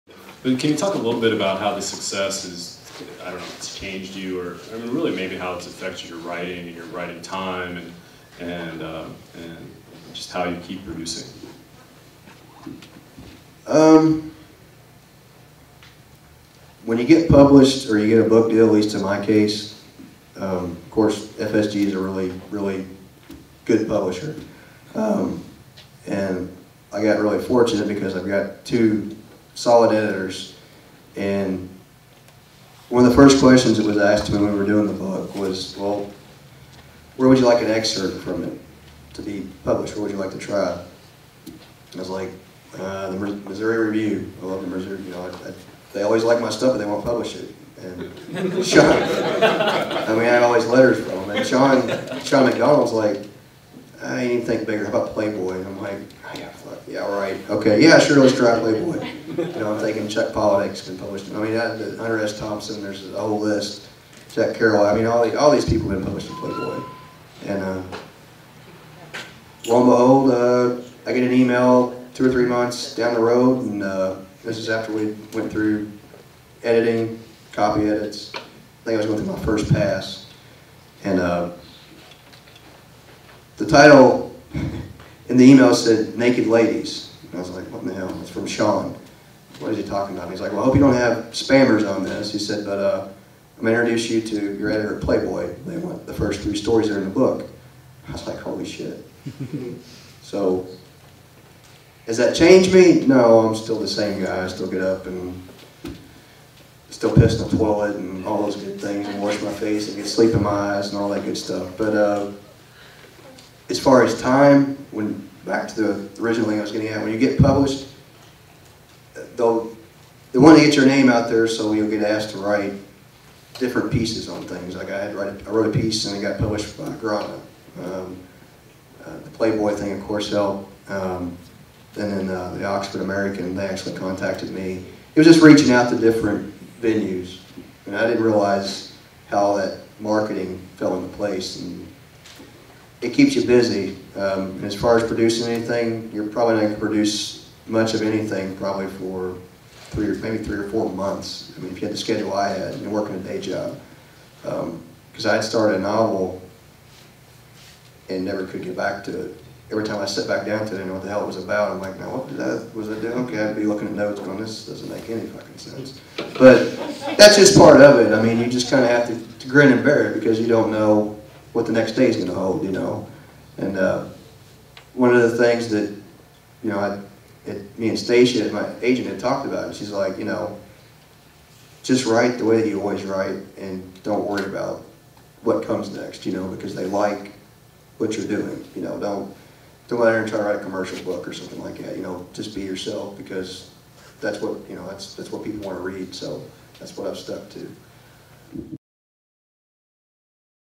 Frank Bill: Dealing with Success
Frank Bill, author of Crimes in Southern Indiana, talks about how he stays productive now that he's experienced success as a writer.

Taped at Big Car's Service Center in Indianapolis.